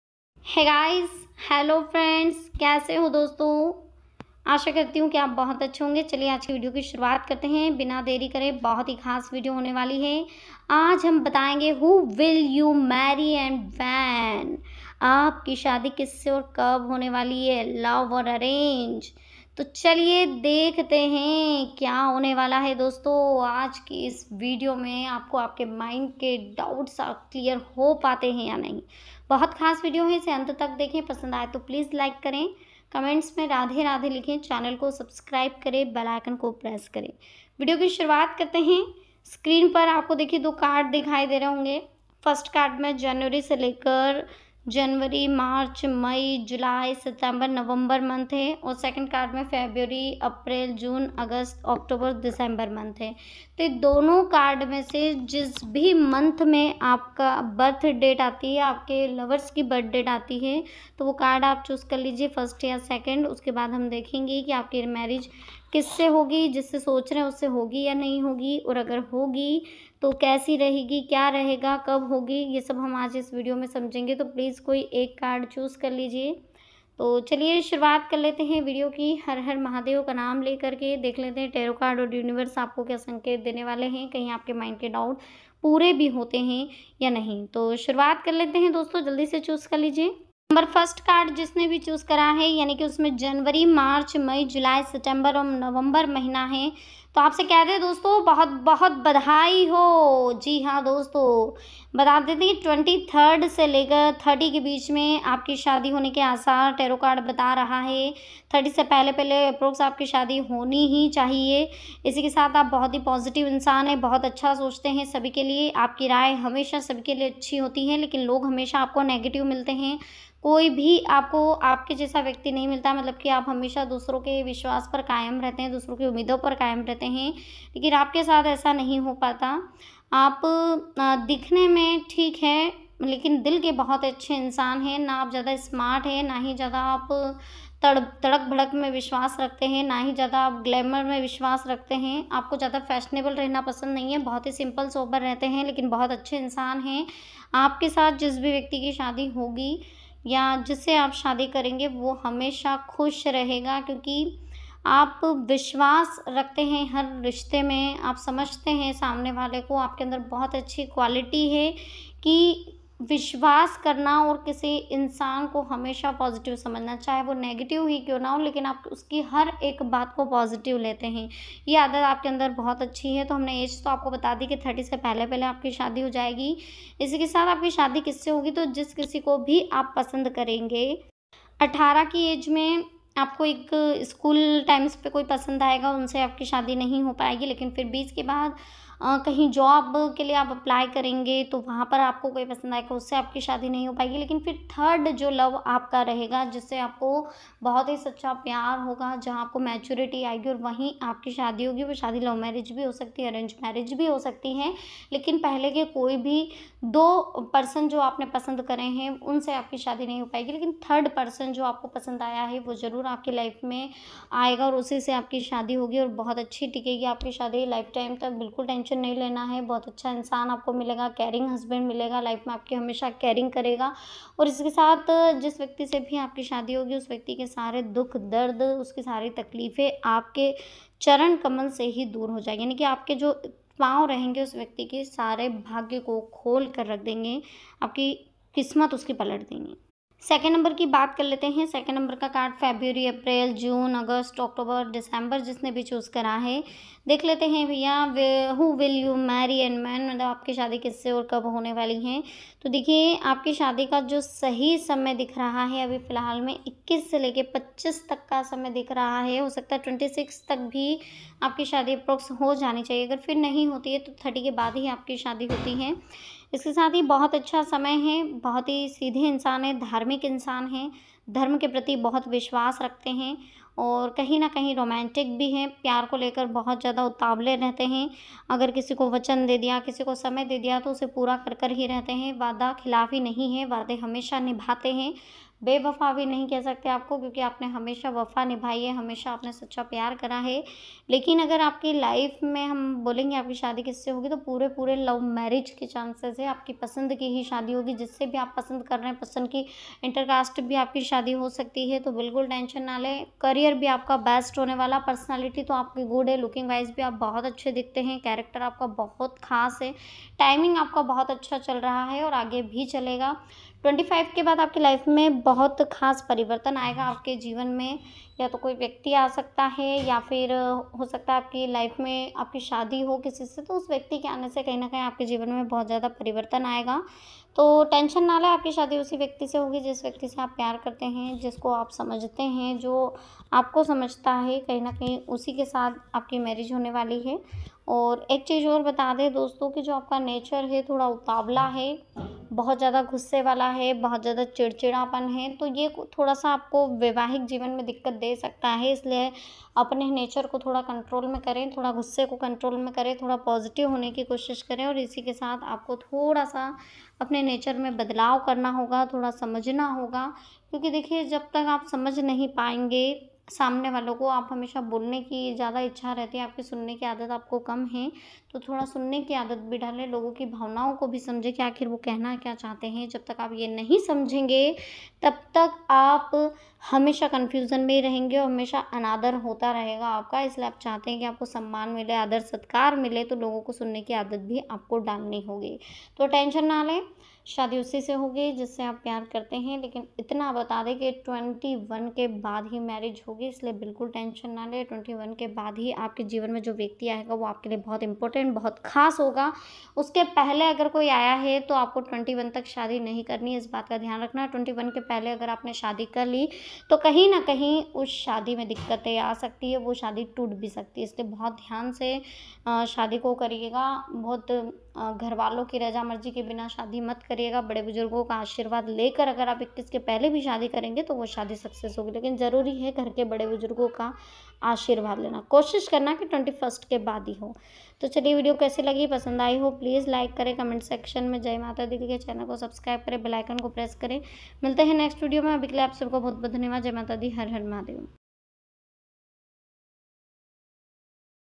Apki Shaadi Kisase aur Kab hogi? Who will you Marry? Destined Partner - Timeless Tarot Reading 🌞💃🕺🌞
past Life relationship connections tarot